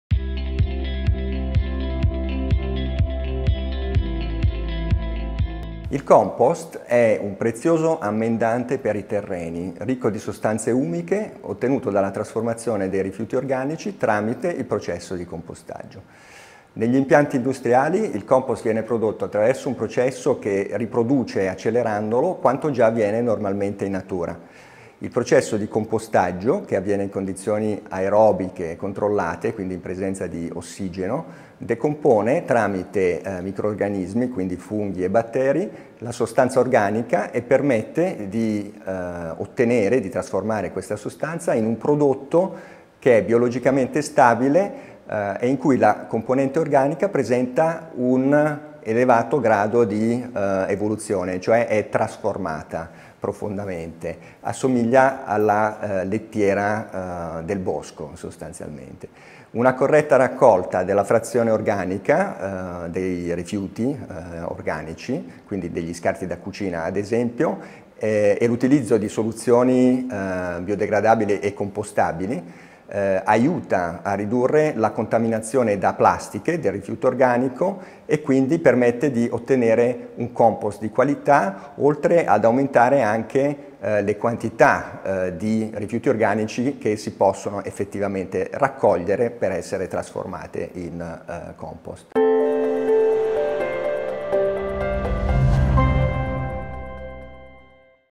WHAT IS COMPOST?
Compost is a valuable soil improver which is rich in humic substances and is obtained from the transformation of organic waste through a composting process.